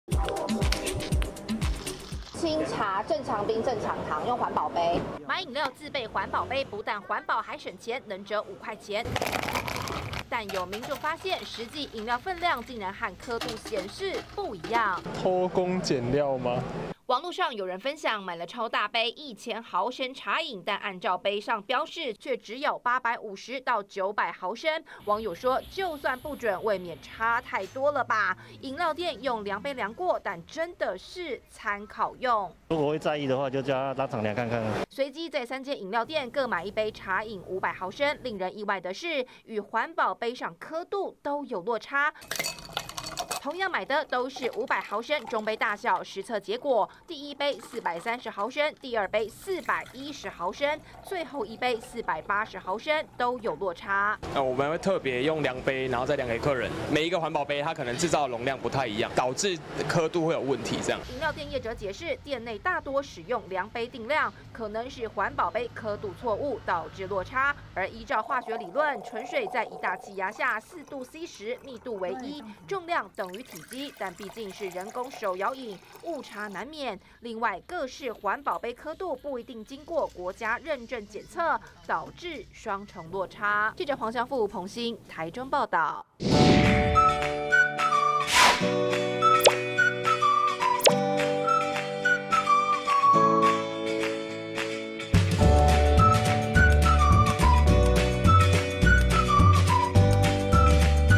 【每日必看】外帶飲料只裝8分滿? 店家揭環保杯"刻度有誤" 20230501
帶環保杯到飲料店，既環保還能現折五元省錢，但有民眾帶環保杯買飲料，意外發現店家怎麼只裝八分滿？店家說，因為環保杯上刻度都不準，真是如此嗎？記者實際測試，用環保杯到手搖飲料店購買茶飲，結果如何，以下是我們的追蹤報導。